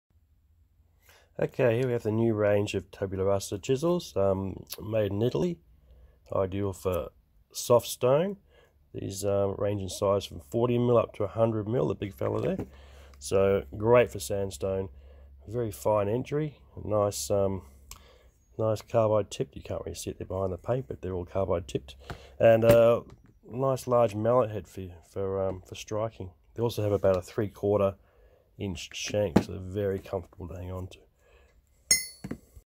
Tabularasa Stone Chisels
Tabularasa Italian stone chisels for sandstone and marble. Mallet headed